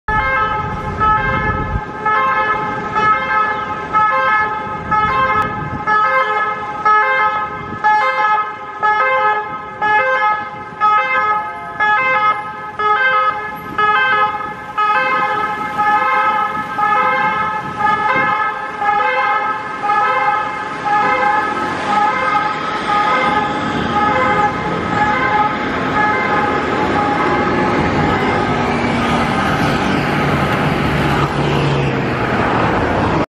Ambulances SEM )))) 84
Ambulància del SEM )))) anant d'urgències per l'Avinguda Diagonal, Barcelona.
Ambulance from the Catalonian SEM )))) service going to an emergency in Av. Diagonal, Barcelona, Catalonia.
Ambulancia del SEM )))) yendo de urgencias por la Av. Diagonal, Barcelona, Catalunya.